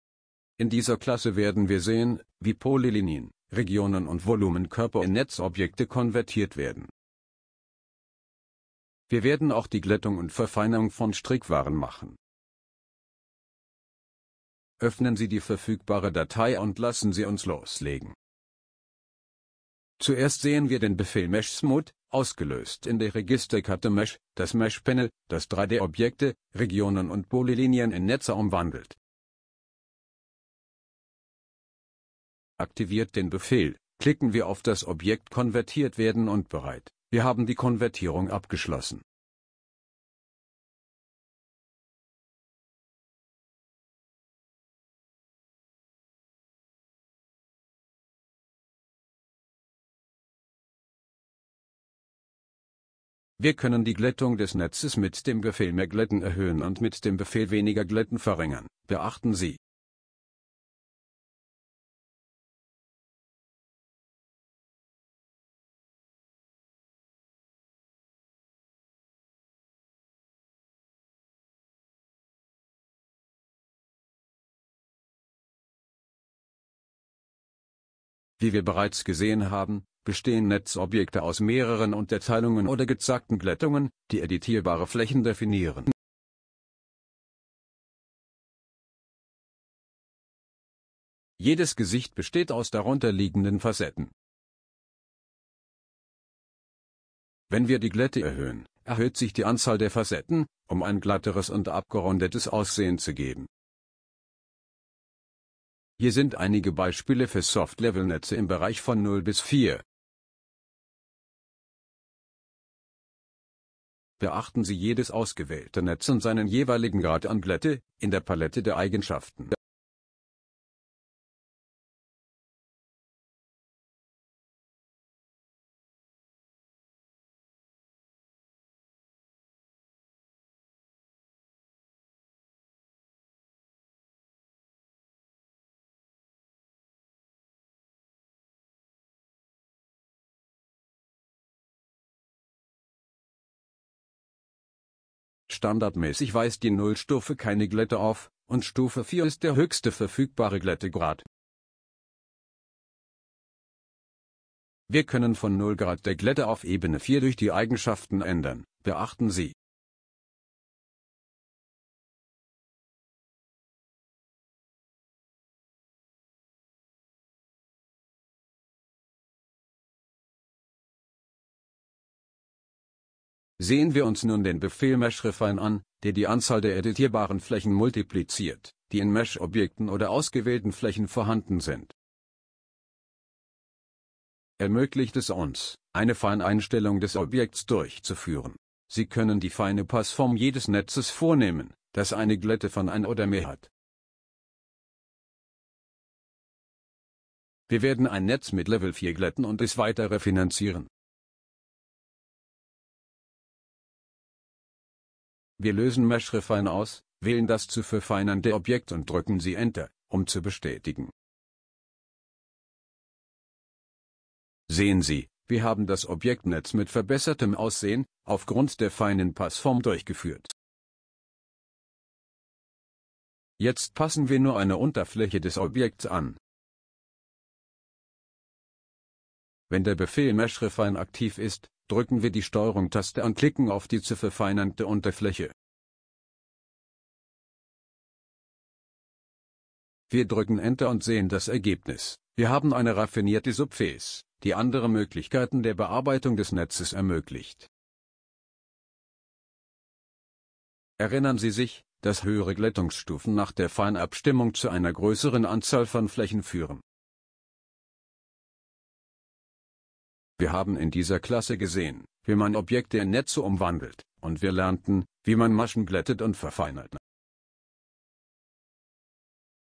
16 - Glätten und Verfeinern von Strickwaren | AutoCAD 2017 -3D Modellierungstechniken
In dieser Klasse werden wir sehen, wie Polylinien, Regionen und Volumenkörper in Netzobjekte umzuwandeln. Wir werden auch das Glätten und Verfeinern von Maschen durchführen.

Komplette Kurse wie AutoCAD, SolidWorks, Inventor und andere, von Basic bis Advanced, finden Sie

Eine großartige Investition für Ihr Wissen.
Profitieren Sie von Sonderpreisen.